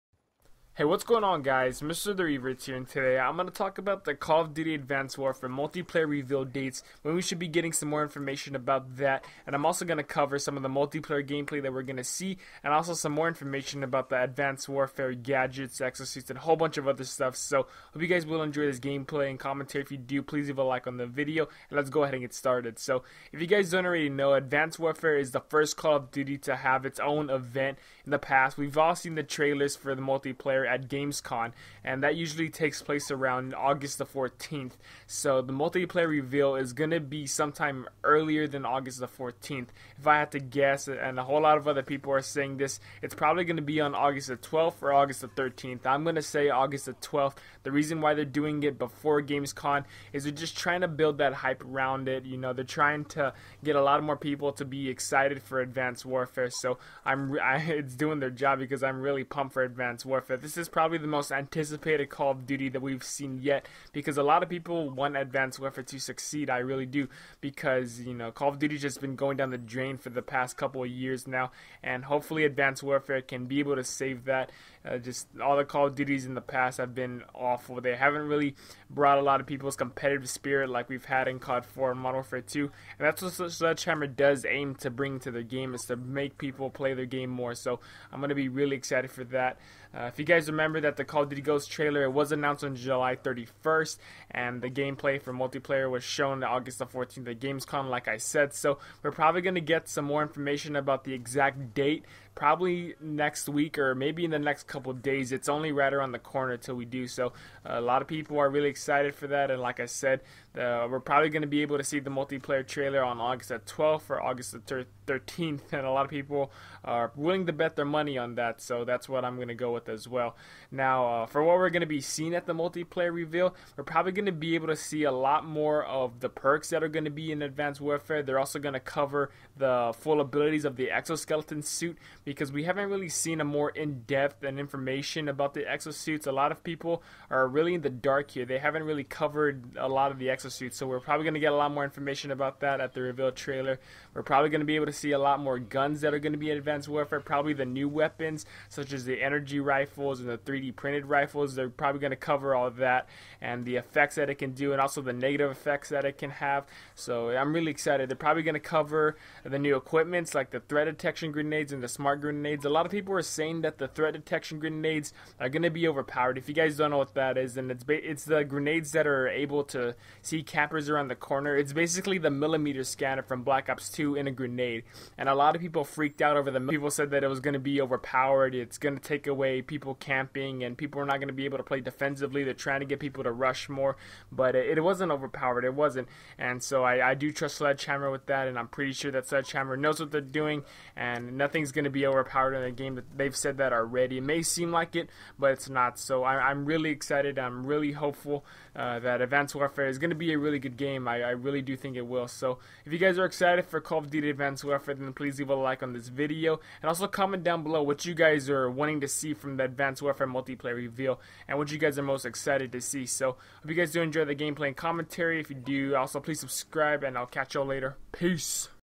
"Call Of Duty Advanced Warfare Multiplayer Gameplay" Dates & More Info!!
Here are some information about the call of duty advanced warfare multiplayer trailer! These are the possible dates & more info!